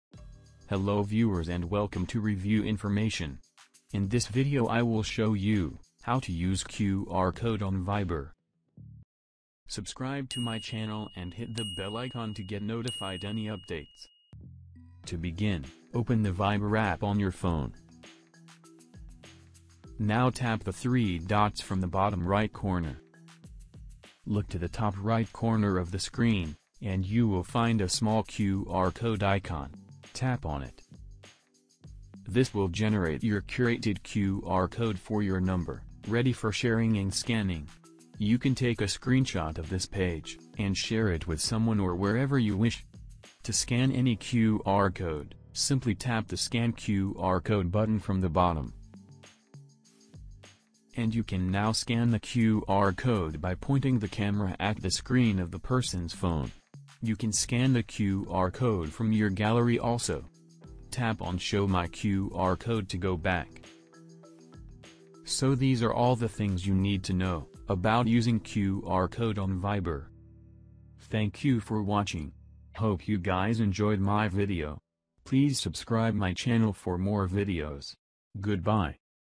How to Use QR Code on Viber
In this video I will show you, How to Use QR Code on Viber.